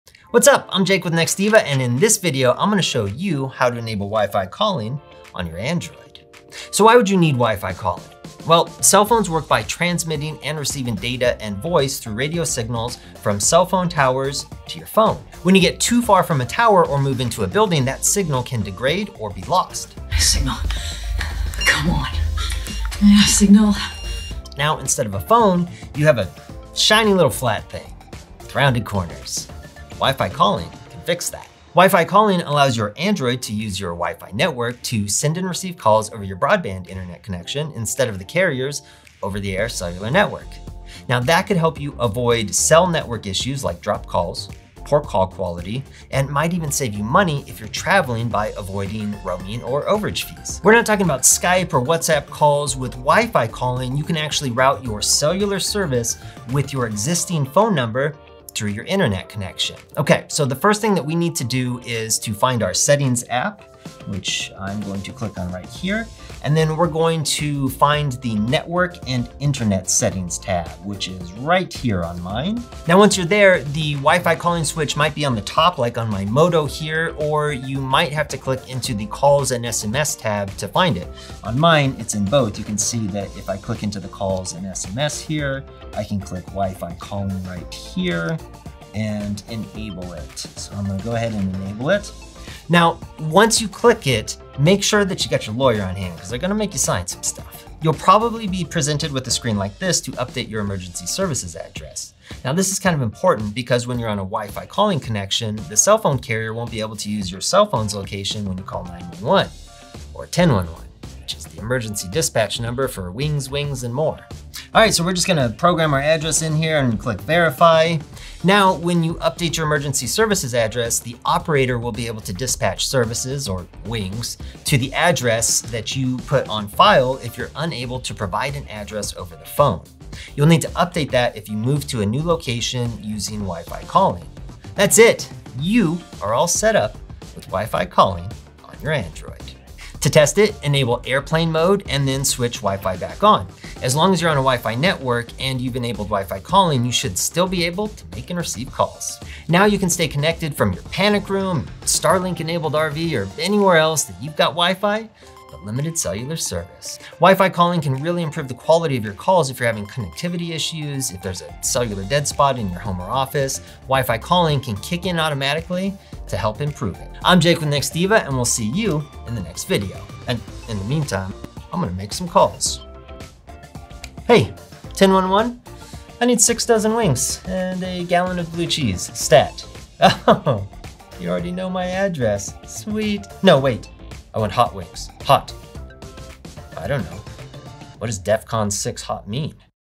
How to Enable WiFi Calling on Your Android (Quick & Easy Setup!)
In this video, we're diving into how to ENABLE WiFi calling on your Android phone. 🔥

📶 Why use WiFi Calling?
No more dropped calls 📞
Better call quality 🎧
Save $$$ on roaming fees 💸
Make calls using your existing number (not Skype or WhatsApp!) 📱

We’ll show you exactly how to set it up so you can make and receive calls over WiFi—whether you're at home, in a dead zone, or even on a WiFi-only connection like Starlink! 🌍

👣 Steps Covered:
1️⃣ Open Settings
2️⃣ Find Network & Internet
3️⃣ Enable WiFi Calling
4️⃣ Update your Emergency Services Address

(Important! 🚨)
💡 Pro Tip: If you’re having cell signal issues, WiFi calling could be your game-changer! Say goodbye to dropped calls and poor quality.

Catch you next time… gotta make some calls. 💪📞

Introduction - 00:00:00
Why Wi-Fi Calling? - 00:00:06
Benefits of Wi-Fi Calling - 00:00:41
Setting Up Wi-Fi Calling - 00:01:00
Testing Wi-Fi Calling - 00:02:20